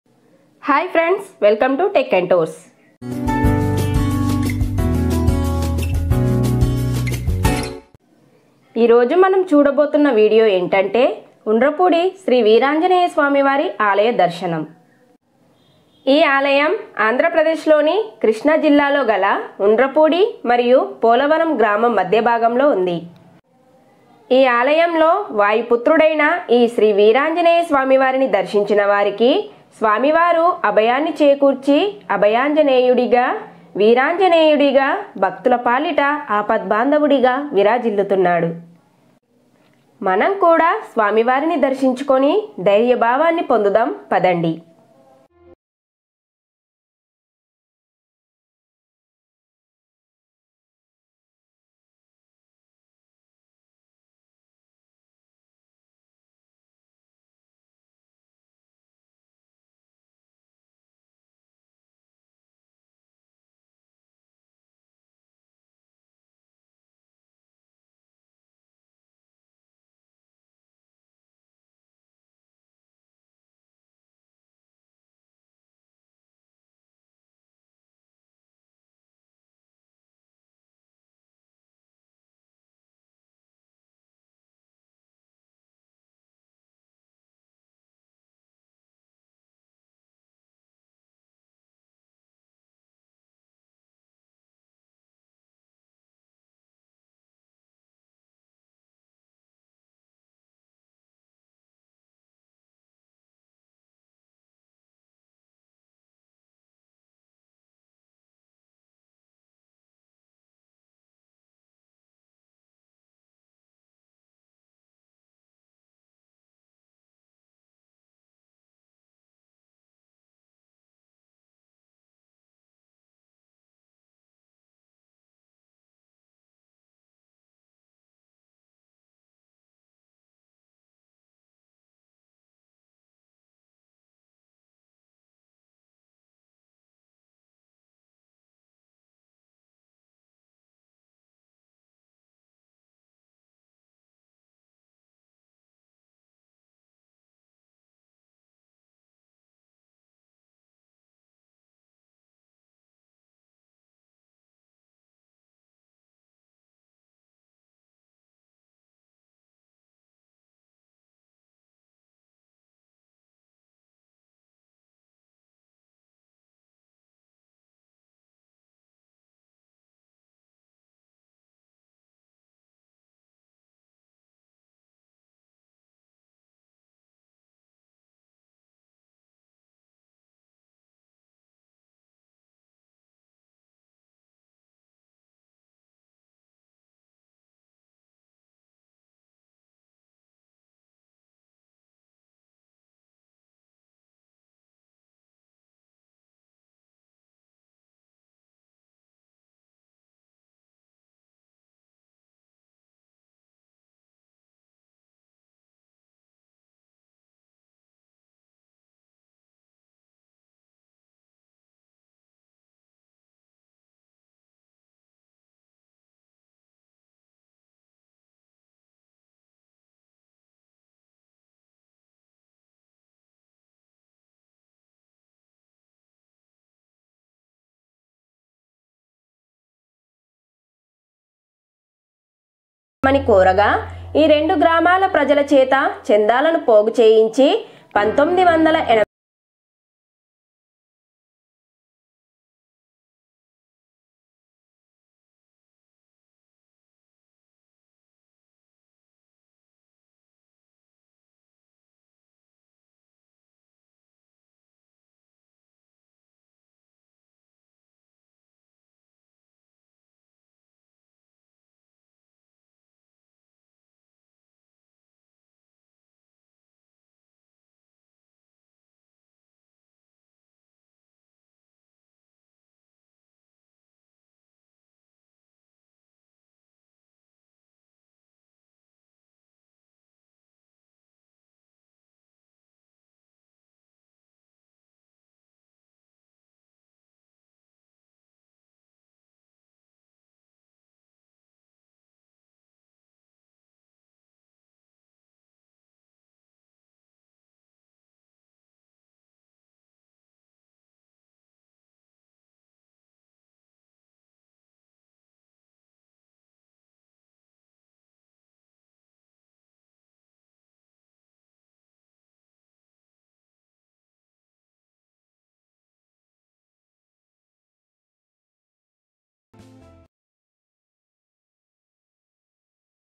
ఉండ్రపుడి పోలవరం శ్రీ వీరాంజనేయస్వామి వారి ఆలయం||Undrapudi Polavaram Sri Veeranjaneyaswamy Temple .
శ్రీ వీరాంజనేయ స్వామి ఆలయం ఆంధ్ర ప్రదేశ్ , కృష్ణా జిల్లా , పామర్రు మండలం , ఉండ్రపూడిలో ఉంది . ఈ ఆలయం వీరాంజనేయ స్వామికి చేయబడింది .
ఈ ఆలయాన్ని  నాలుగు దశాబ్దాల క్రితం పోలవరం మరియు ఉండ్రపుడి గ్రామస్థులు కలసి నిర్మించారు.
Back ground music -Bensounds.com